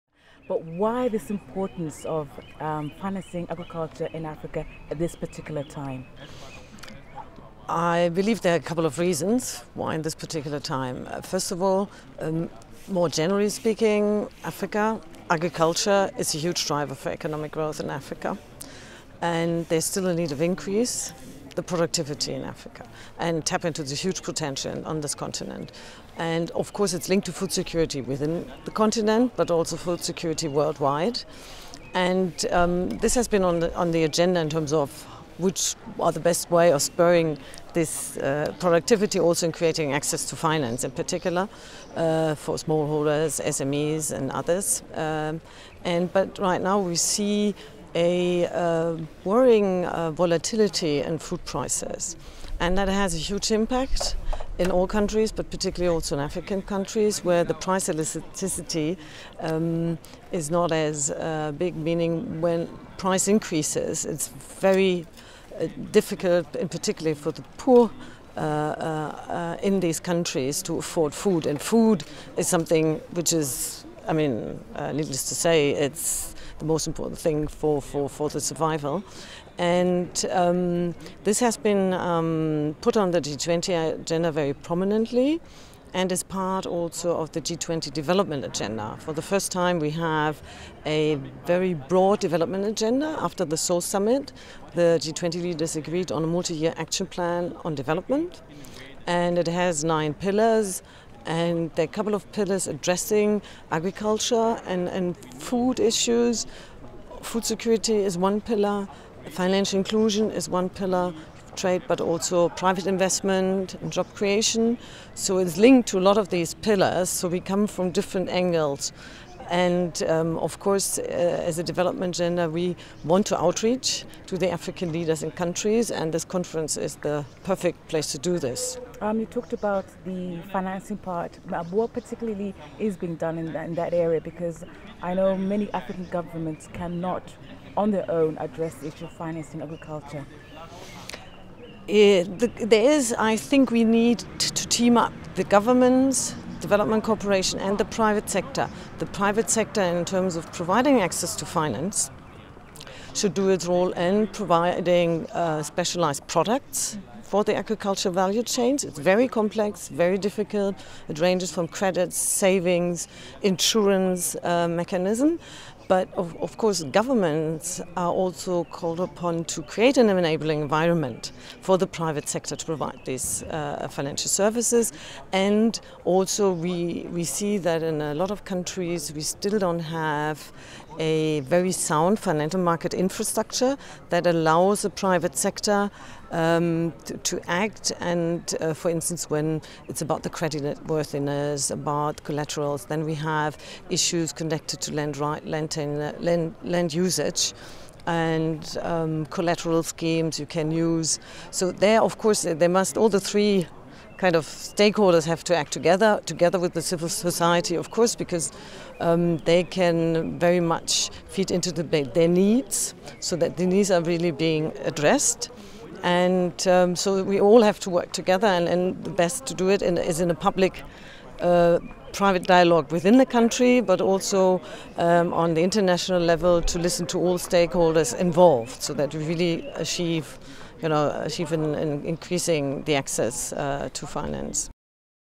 Interview with Susanne Dorasil, German Ministry for Economic Cooperation and Development (BMZ)
Interview with Susanne Dorasil, head of economic policy/financial sector division at the German Ministry for Economic Cooperation and Development (BMZ) and chair of the G20 Agrifinance, during the conference "Zipping Finance and Farming in Africa", which took place in Kampala, Uganda, on June 29 and June 30, 2011.

Interview by: Yvonne Msemembo